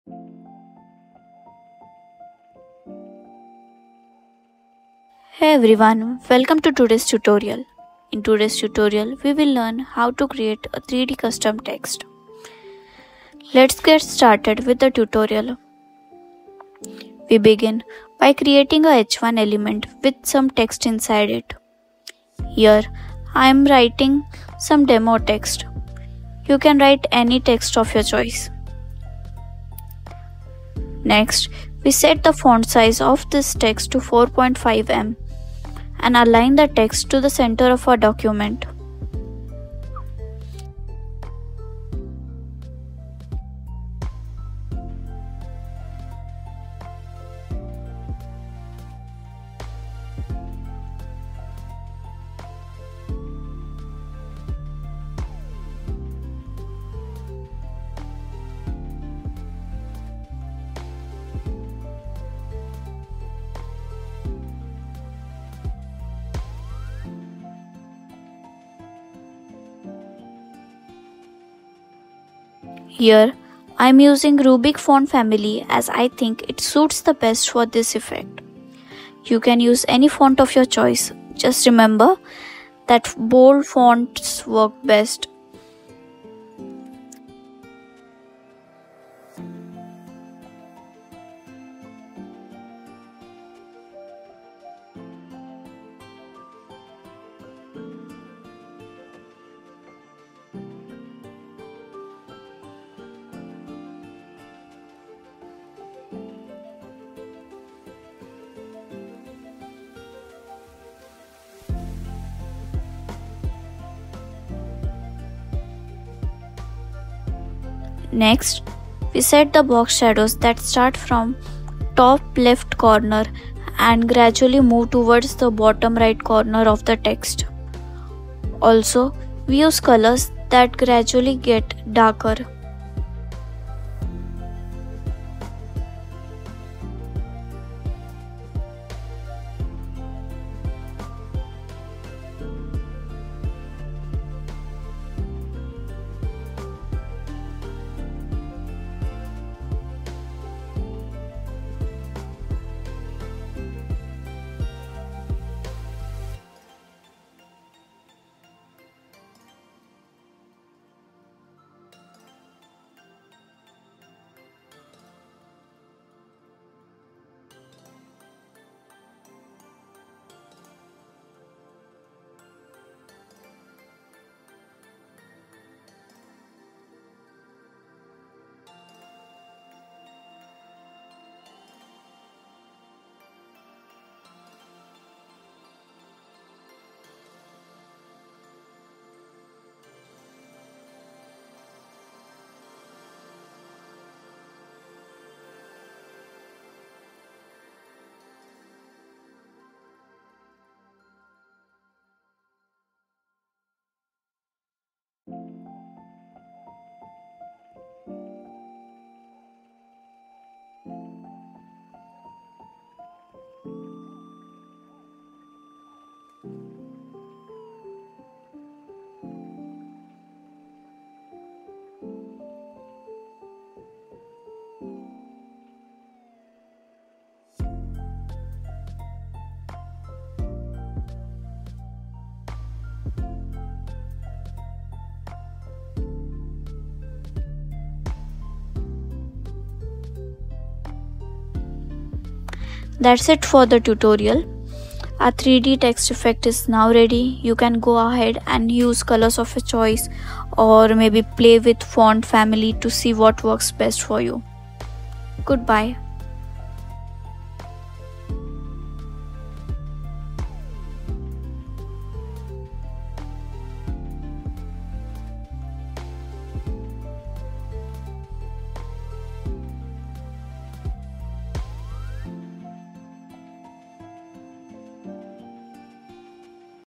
3d Text Effect | Pure CSS Tutorial
Learn how to create custom 3D text effect with HTML and CSS. You can easily edit this text to any text of your choice and customize it the way you want.